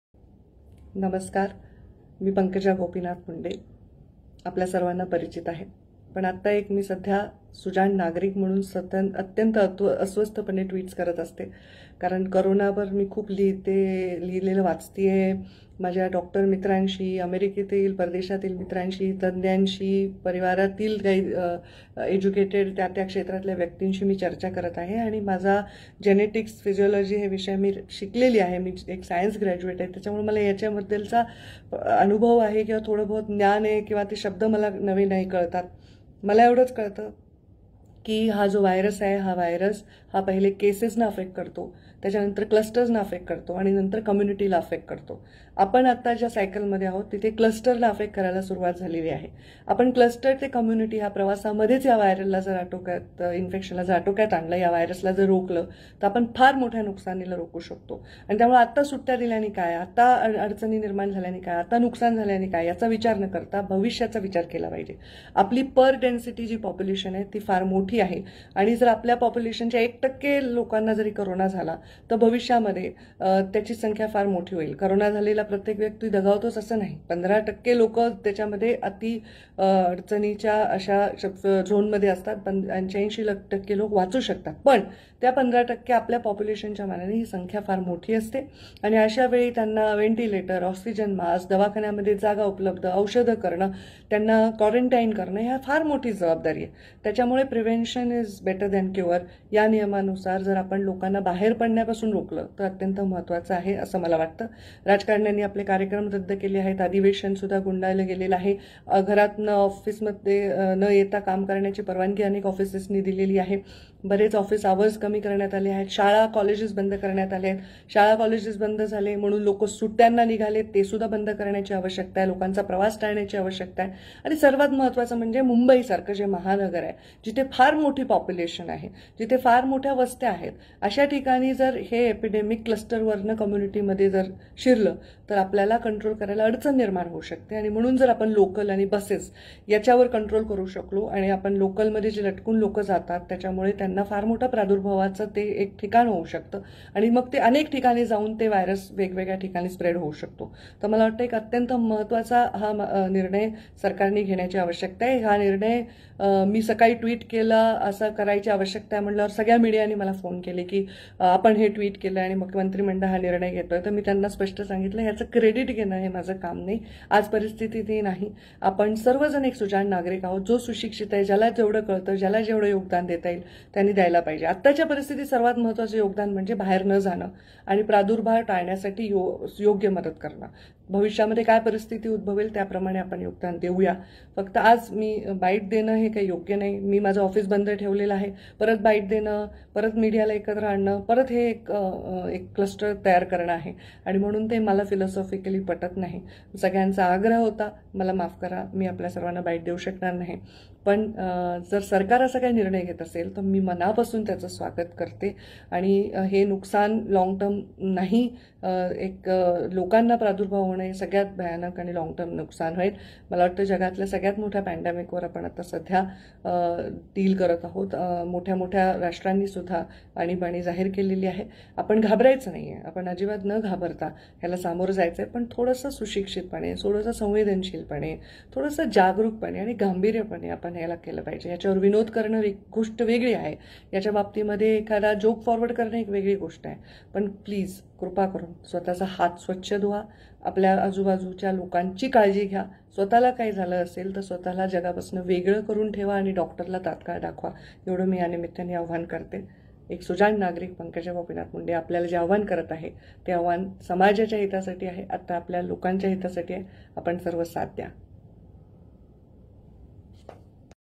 Pankaja Gopinath Munde
एक सामाजिक आवाहन - कोरोना विषाणूपासून आपले, आपल्या कुटूंबाचे आणि आजूबाजूच्या सर्व व्यक्तींचे संरक्षण करण्यासाठी..